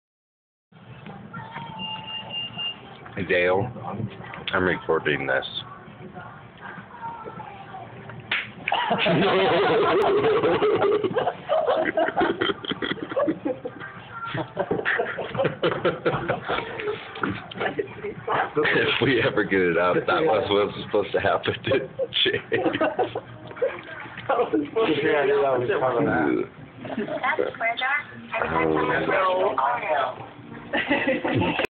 James slaps dale
James getting dale back, Aww brothers